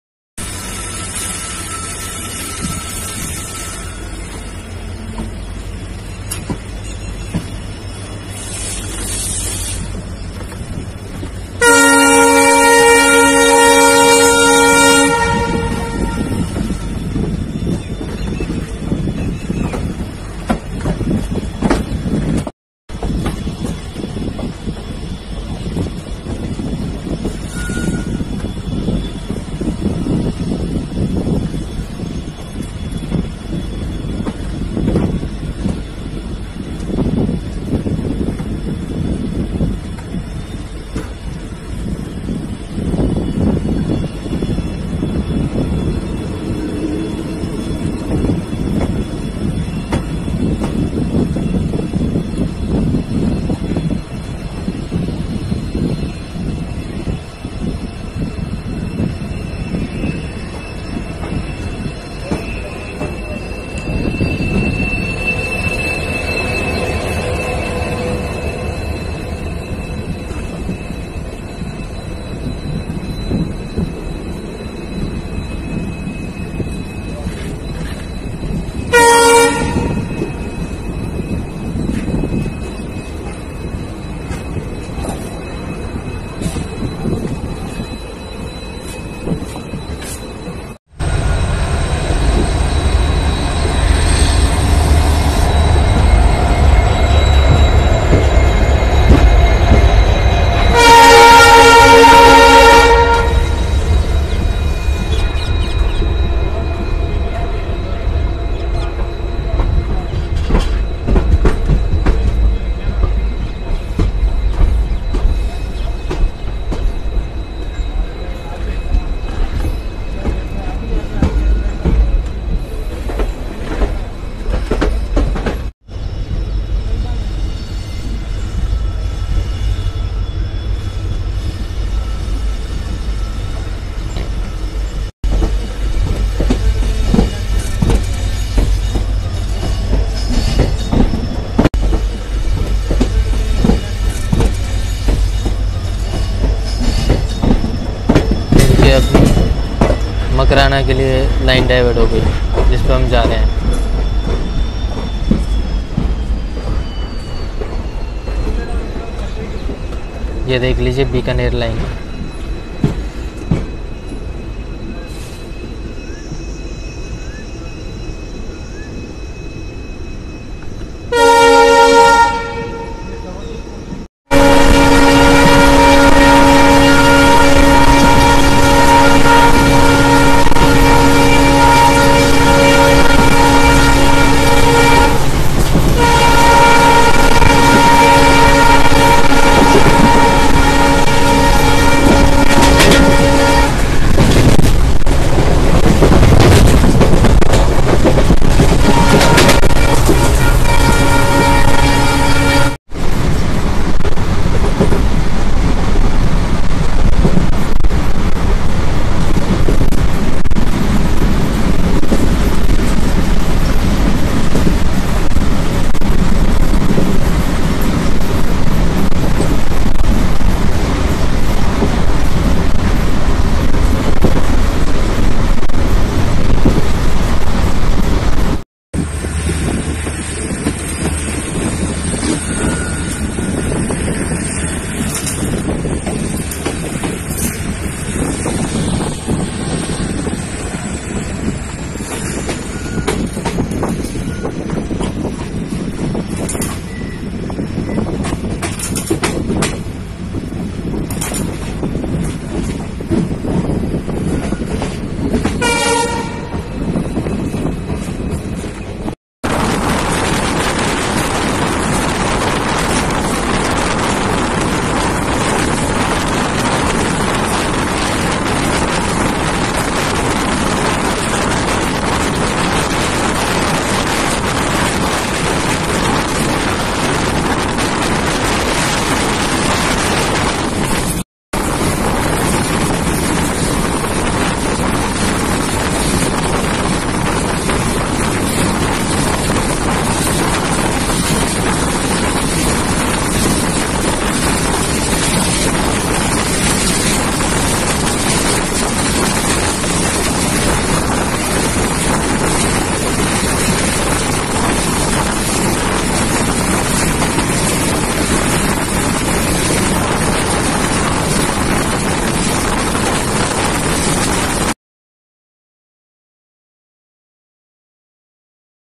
04645 Jaisalmer Jammu Tawi Shalimaar Express Journey Merta Road Junction To Makrana Doubling Update.
Thanks Rail Lover MJ Bhai